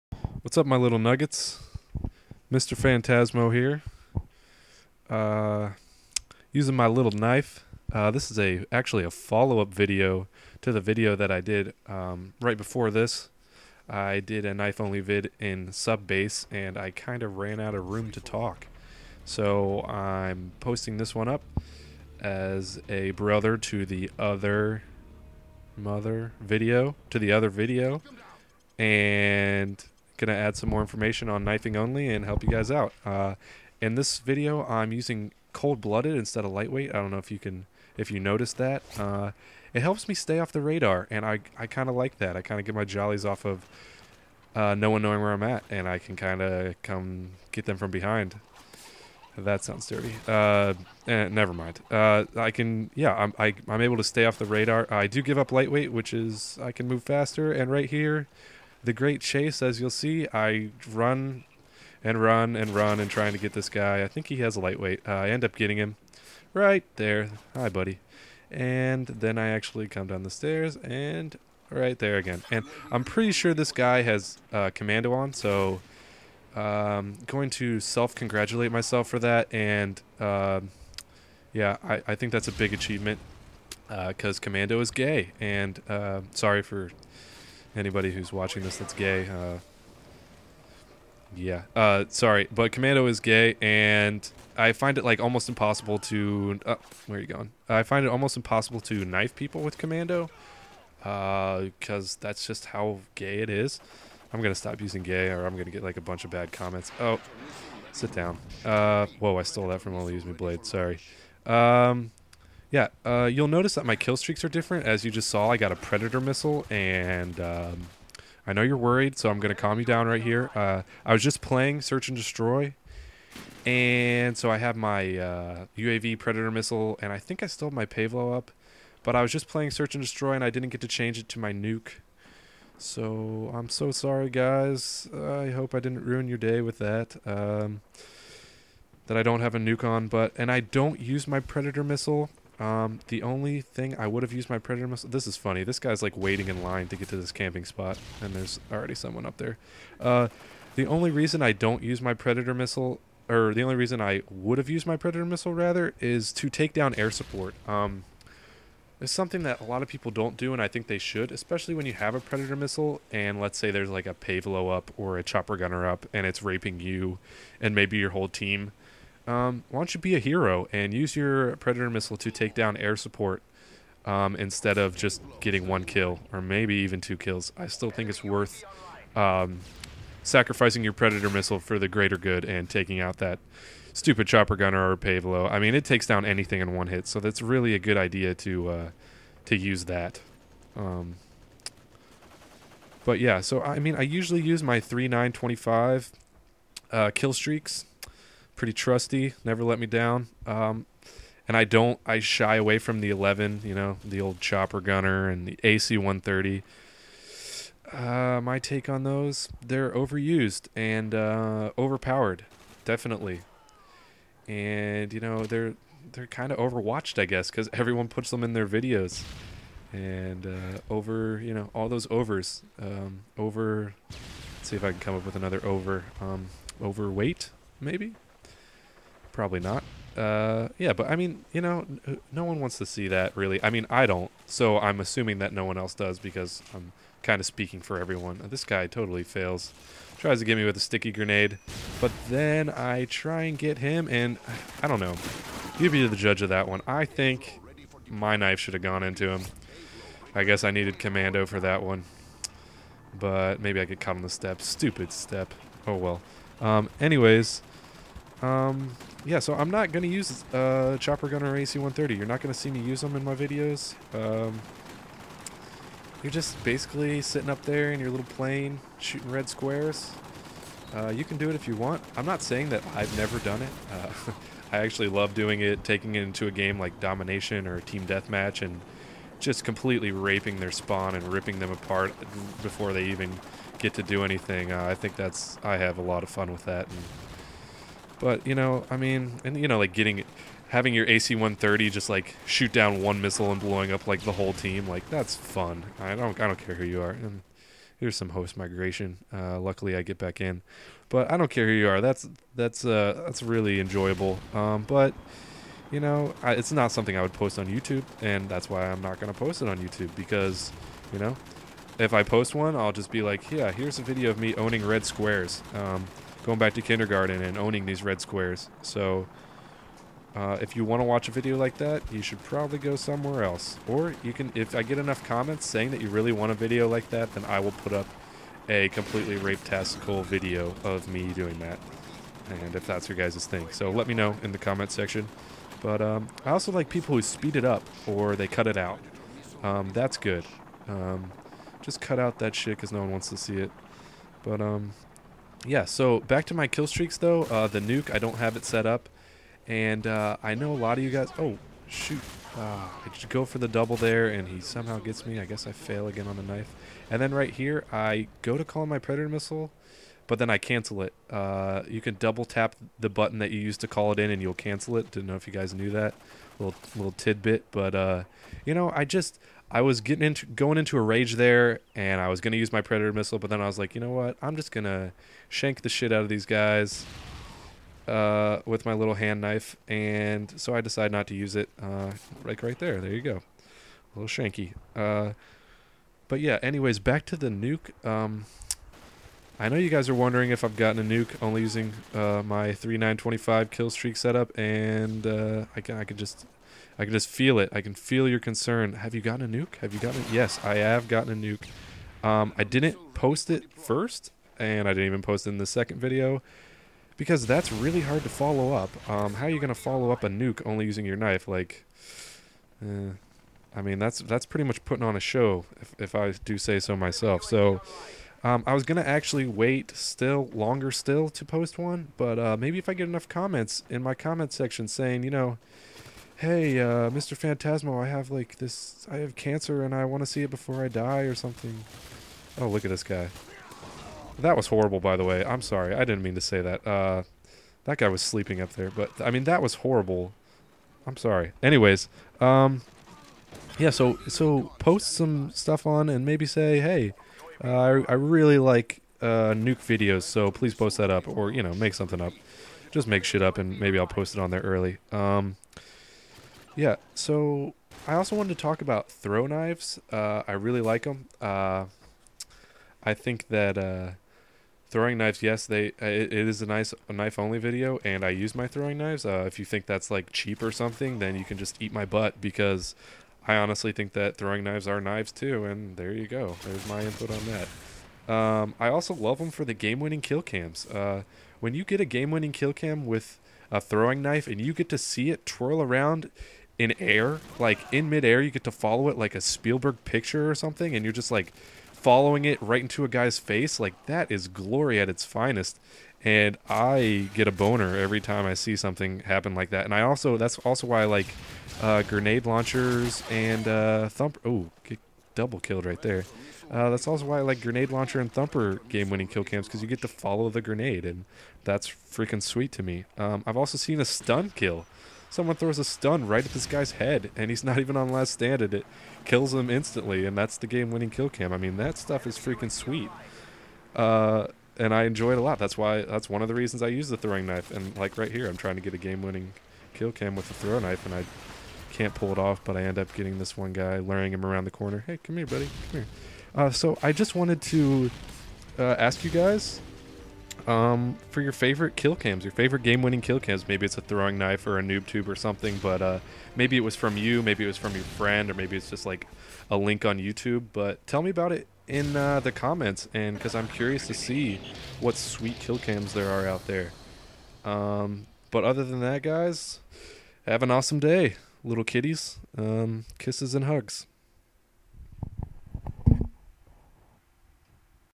One DANGEROUS Dagger | The Origin of What's Up my Nuggets
What up my nuggets?! haha Mister Fantasmo here giving you the promised follow up vid to my ONLYUSEmeBLADE tribute. Just continuing to spread my infinite wisdom in this one as far as knifing only goes because I just love sharing. Anyways check it out tell me what you think and just remember ( .)(. )